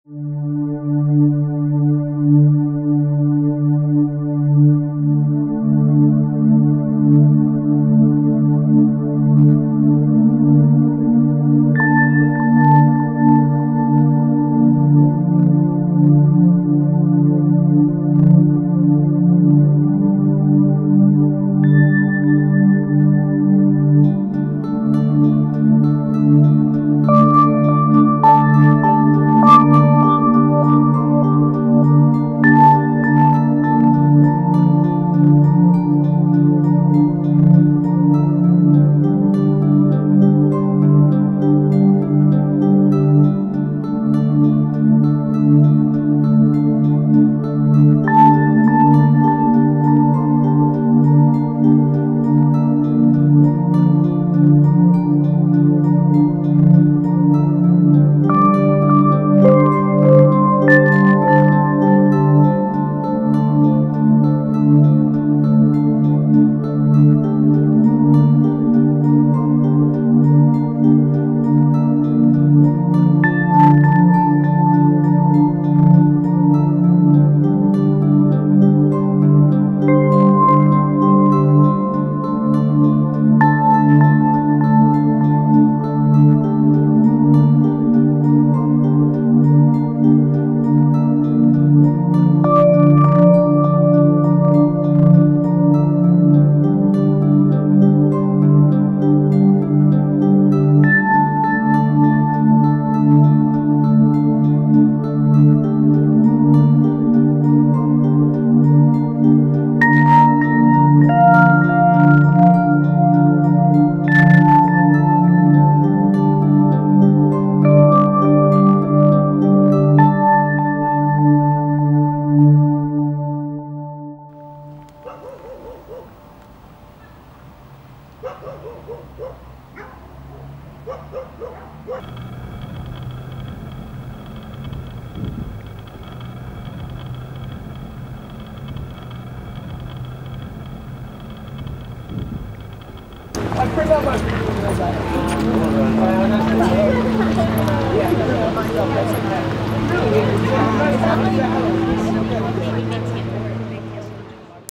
Are You Listening?
Human trafficking panel March 6, 2013 4-6pm Sid Rich Lecture Hall 2
Panel:
TCU Department of Political Science
Cook Children's Hospital
Mosaic Family Services
Traffic 911
ACH Child and Family Services
*Composition by Will Wood